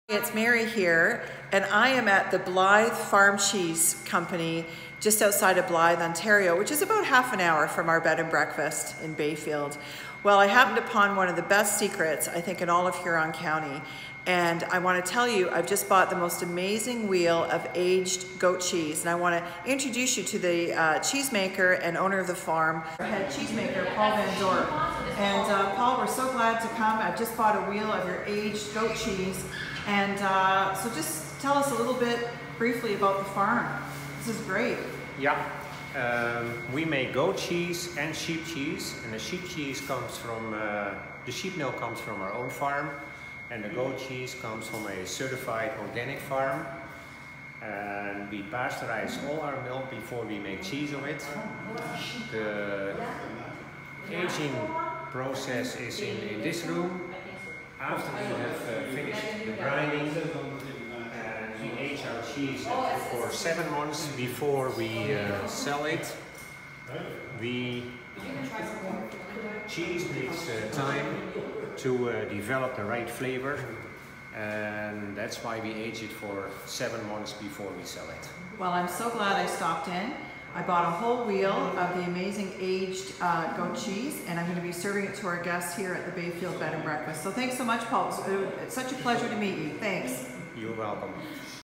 Blyth Cheese Company
🌺 This is a new place I discovered in Huron County 30 mins from our Bayfield Boutique Bed & Breakfast

👉 What a find if you love cheese as much as I do!

😊 I definitely think this is a wonderful taste experience and to go there when visiting the area!

Mary XO 💕